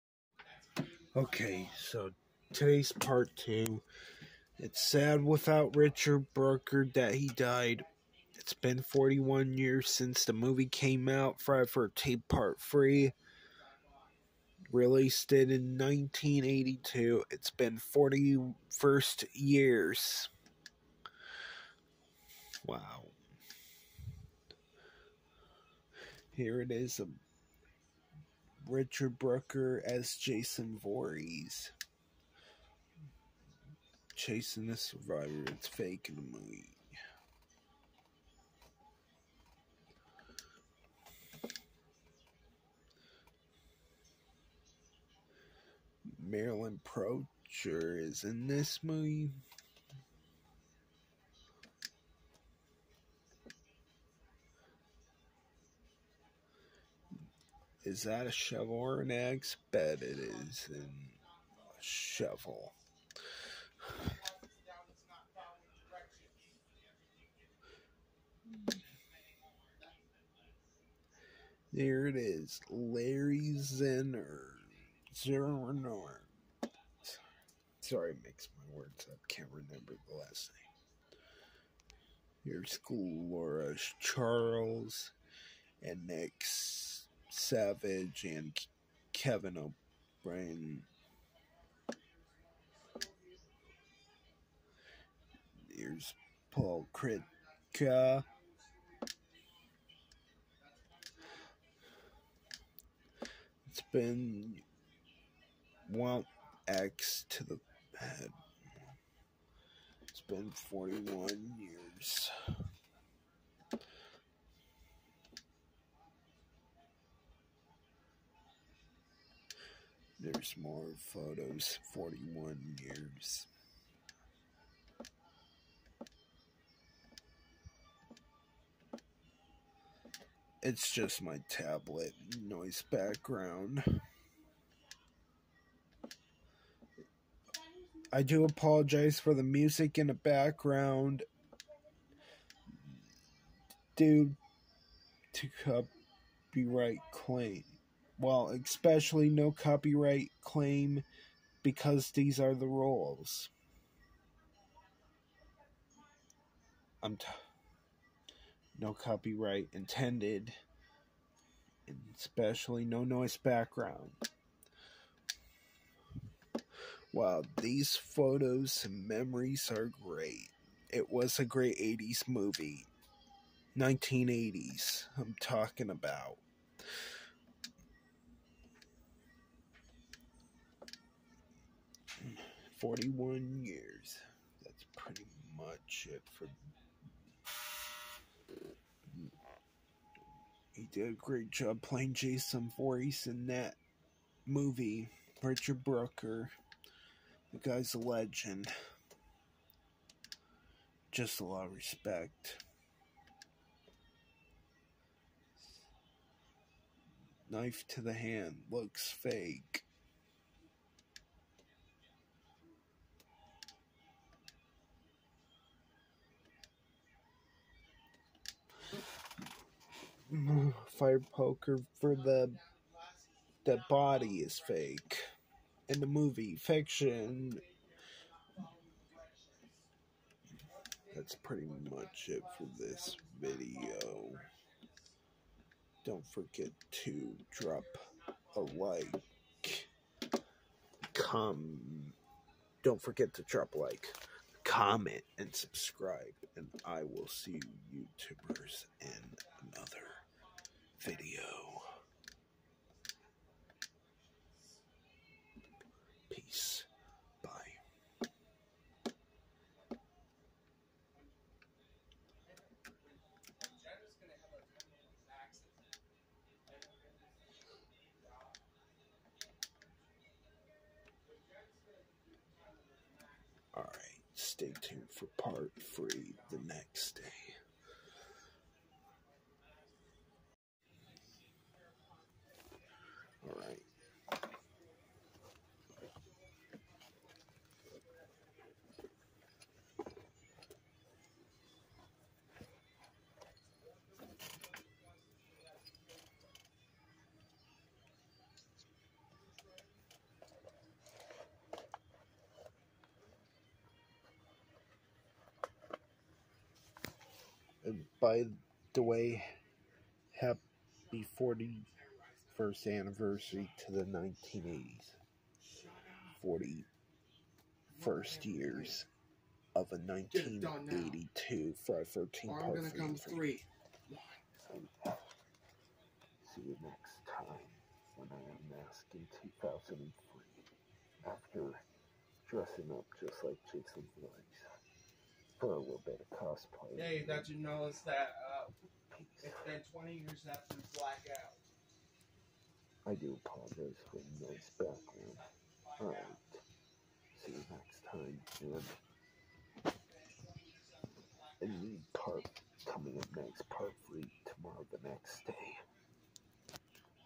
Friday the 13th Part 3 1982 movie 41st first anniversary / 41 years later(Part 2)
yesterday Friday the 13th Part 3 1982 movie 41 years old this year I'm talking about the 1980s Movie it was great so amazing so far